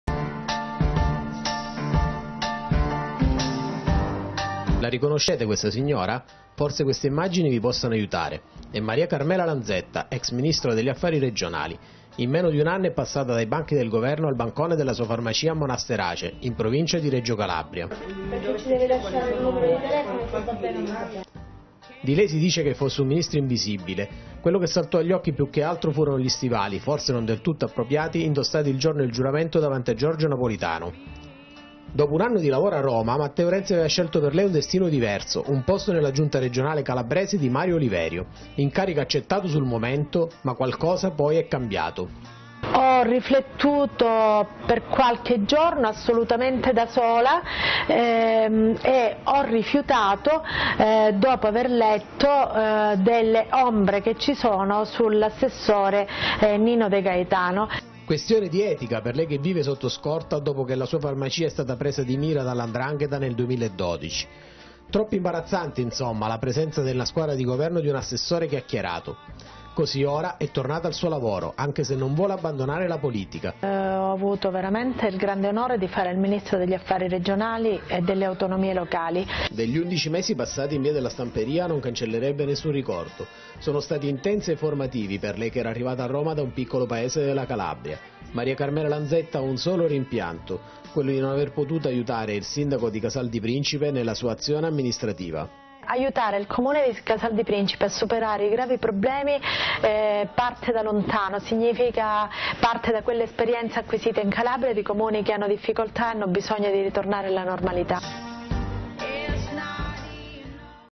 L'ex ministro torna a lavorare in farmacia
La riconoscete? L’ex Ministro per gli affari regionali, Maria Carmela Lanzetta, in meno di  un anno è passata dai banchi del governo al banco della farmacia